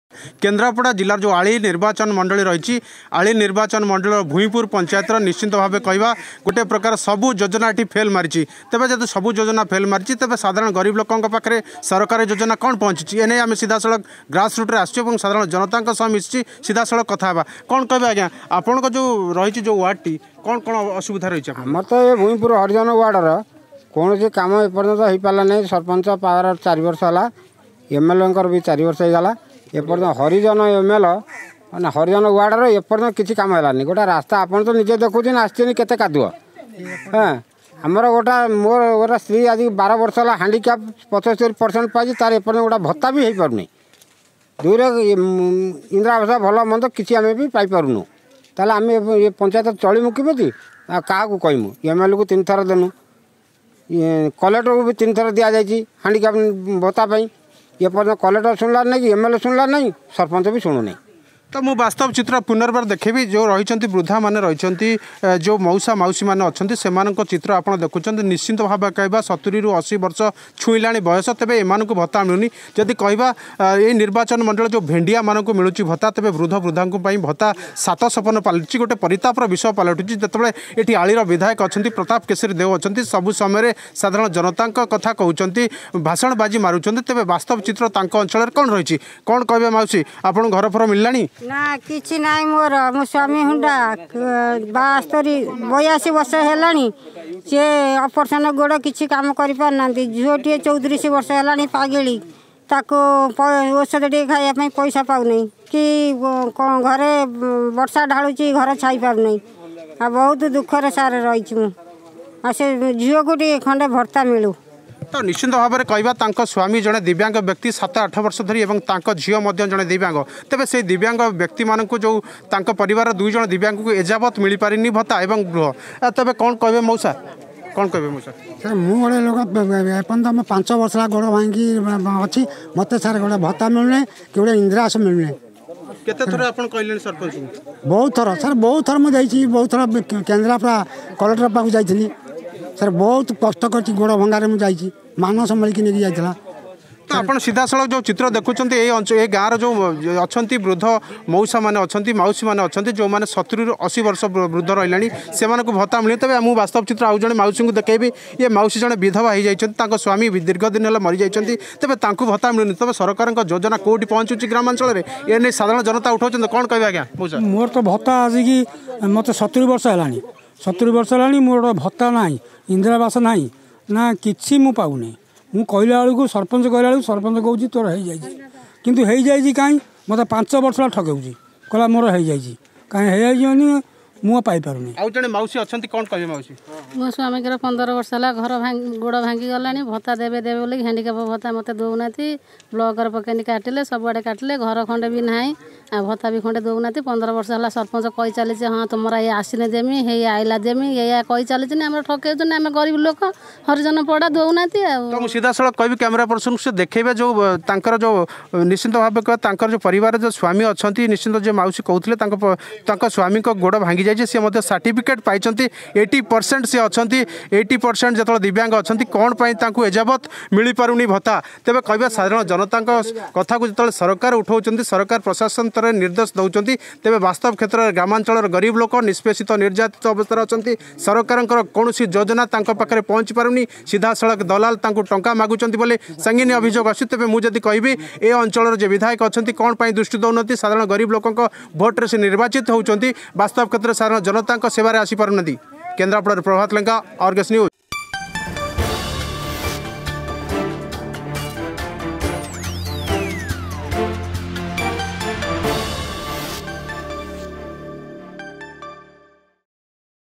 ବାର୍ଦ୍ଧକ୍ୟ ଭତ୍ତା ପାଇବାରୁ ବଞ୍ଚିତ ଭୂଇଁପୁର ପଞ୍ଚାୟତବାସୀ
ଭେଣ୍ଡିଆ ପାଉଛି ଭତ୍ତା ଆଉ 80ବର୍ଷର ବୃଦ୍ଧା ବାର୍ଦ୍ଧକ୍ୟ ଭତ୍ତା ପାଇବାରୁ ବଞ୍ଚିତ. ଏମିତି ଘଟଣା ଦେଖିବାକୁ ମିଳିଛି କେନ୍ଦ୍ରାପଡ଼ା ଆଳି ବ୍ଲକ ଭୂଇଁପୁର ପଞ୍ଚାୟତରେ ଶୁଣନ୍ତୁ ଏନେଇ କଣ କହୁଛନ୍ତି ଗ୍ରାମବାସୀ .